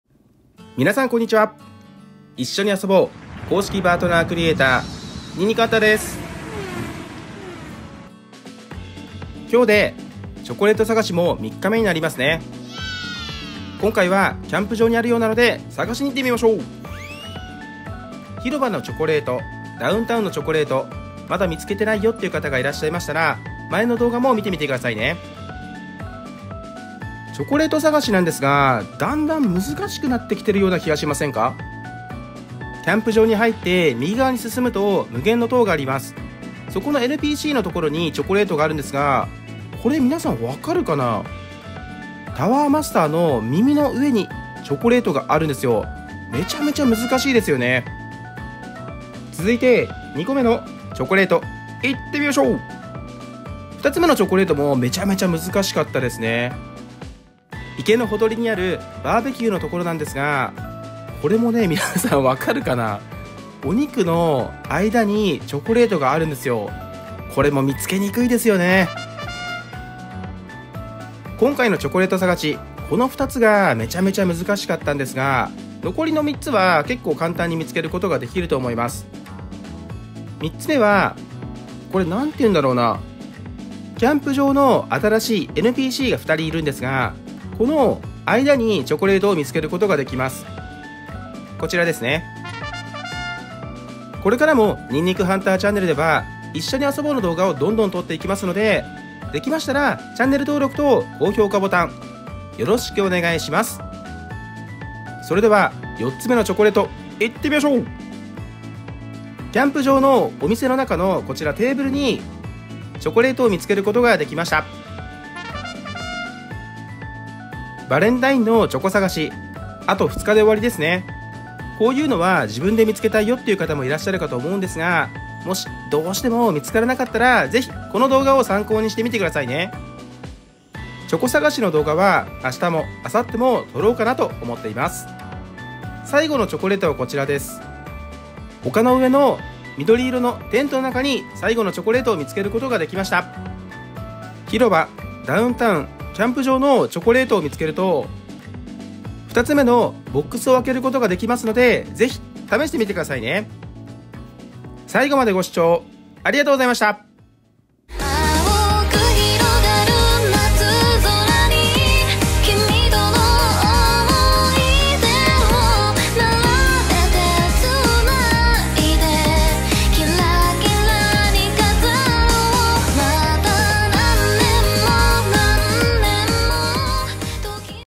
【一緒に遊ぼう】３日目キャンプ場チョコの場所紹介します
チョコレート探し今回はキャンプ場ですねだんだんチョコの場所が難しくなってるような気がします。キャンプ場は範囲が狭いからなんでしょうか？

キャンプ動画おすすめ

【クラムチャウダー】キャンプ飯　元料理人が作る具沢山クラムチャウダー

音楽　魔王魂